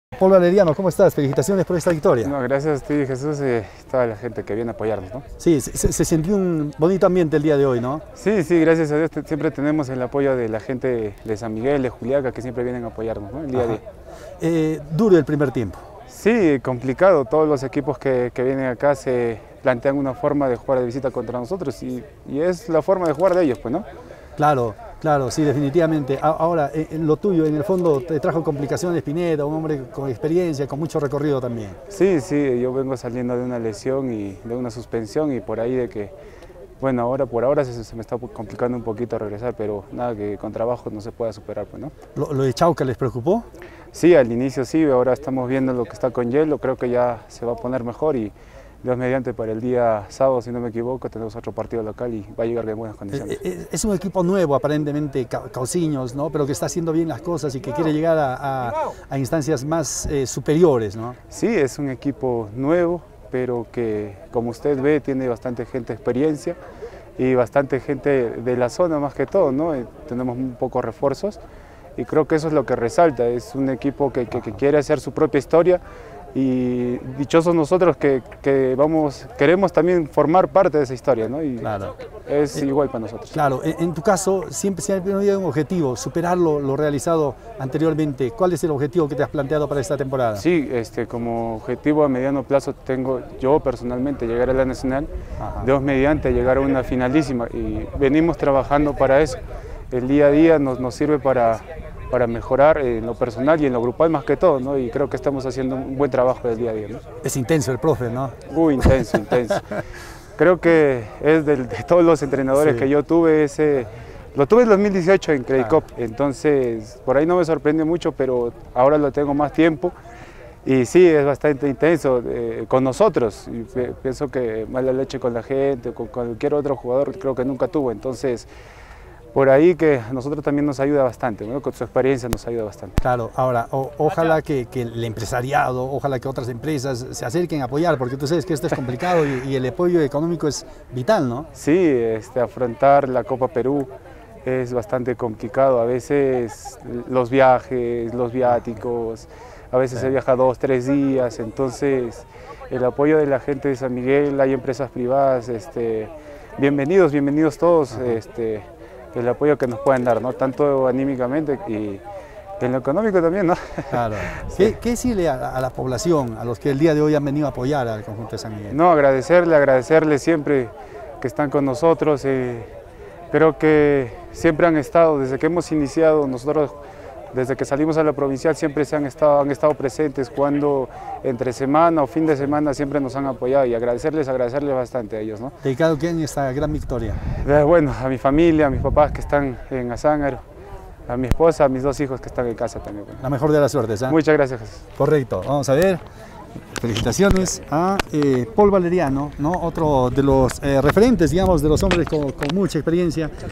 Paul Valeriano Cahusiños 3 Policial Santa Rosa 0 Copa Perú en Puno
Defensa del Cahusiños Paul Valeriano habla de la victoria de su equipo  Cahusiños 3 Policial Santa Rosa 0 por la Copa Perú etapa departamental en Puno, partido jugado en Juliaca